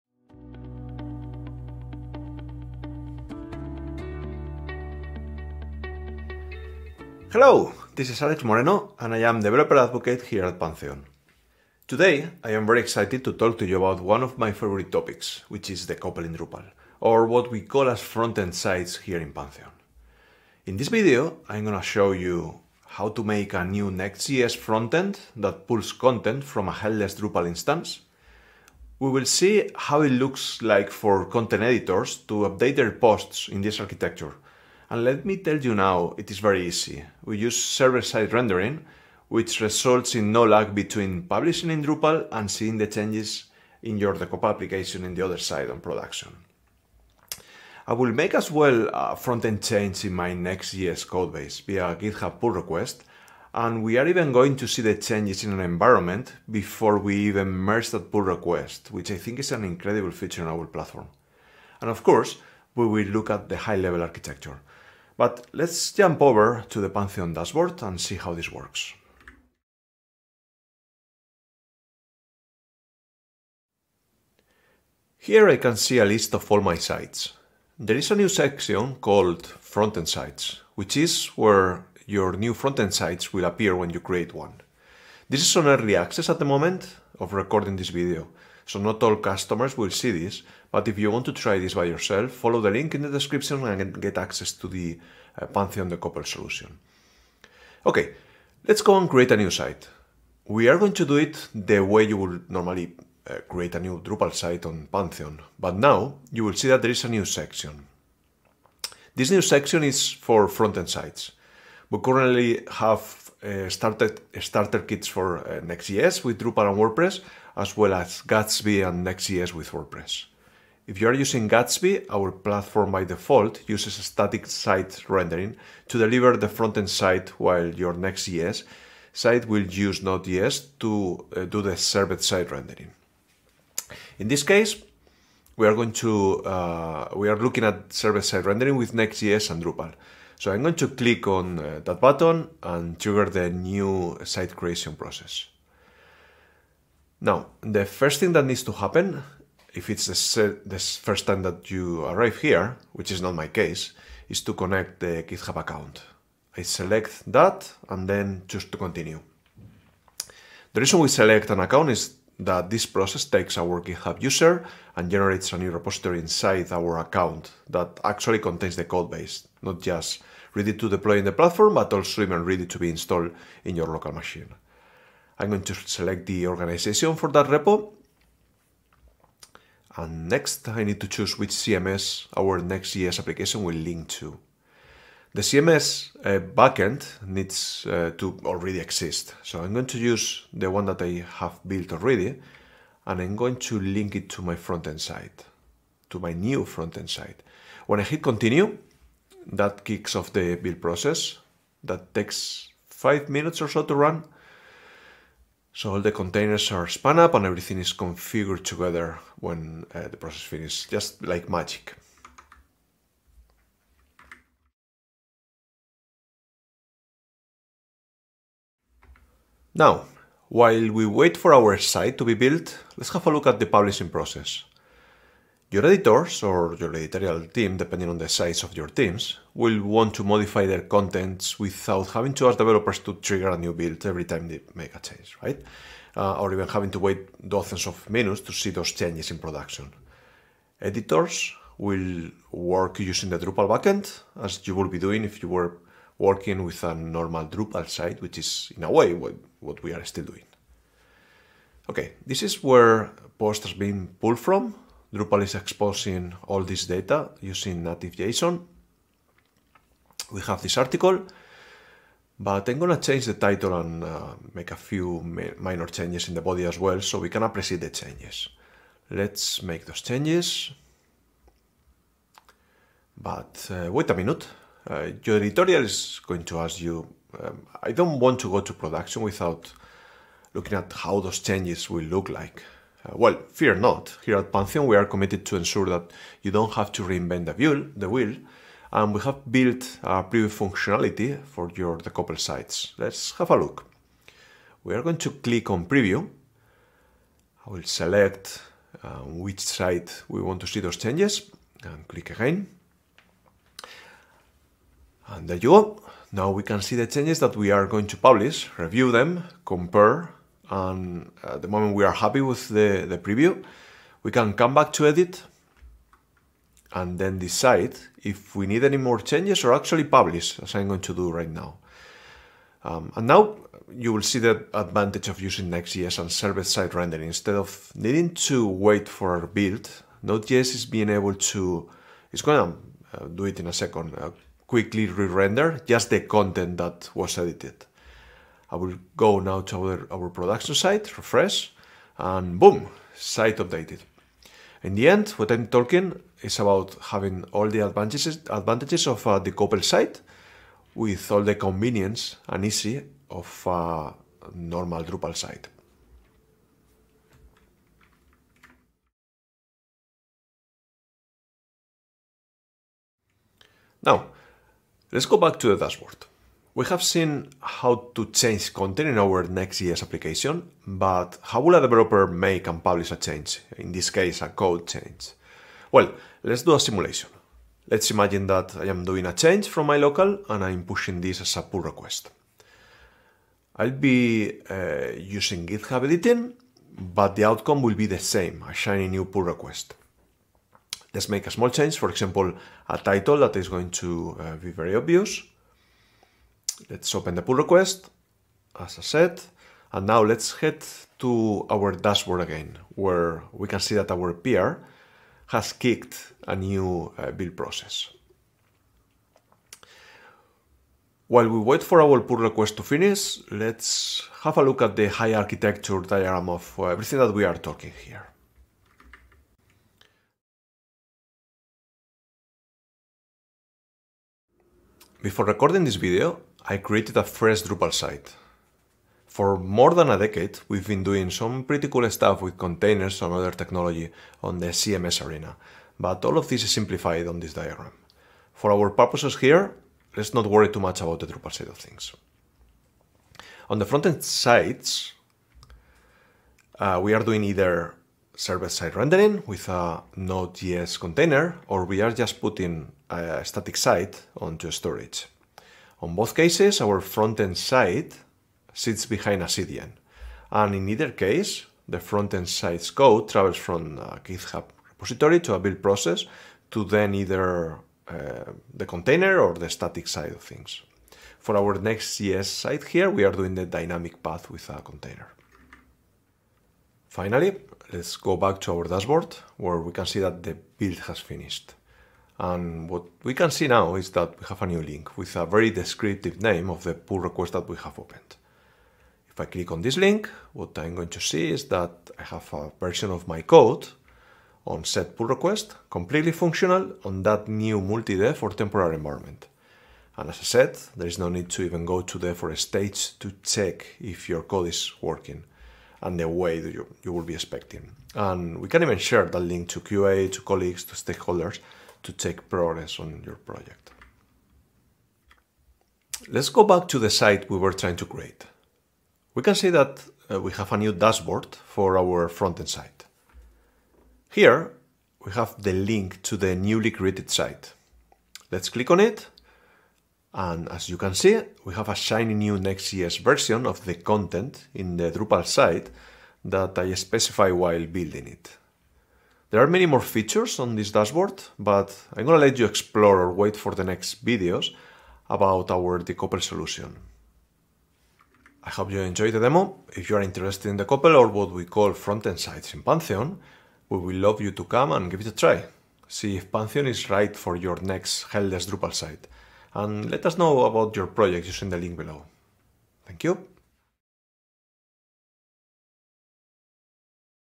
Next.js + Drupal Starter Kit on Pantheon Front-End Sites
Pantheon Developer Advocate, Alex Moreno, walks through Pantheon's Next.js + Drupal starter kit. We aim to balance the needs of developers working through GitHub, content editors who expect instant publishing, and website owners who want back-end and front-end systems to live under one roof. This starter kit is available to anyone with access to Pantheon's new Front-End Sites product.

For more details, read about Pantheon's approach to starter kits

Chapters:
0:00 - Intro
1:10 - Making a New Next.js Site on Pantheon
3:36 - Instant Content Publishing Through Drupal
6:00 - Changing Next.js Code via Pull Request
7:04 - Front-End Sites Architectural Overview
9:09 - Next Steps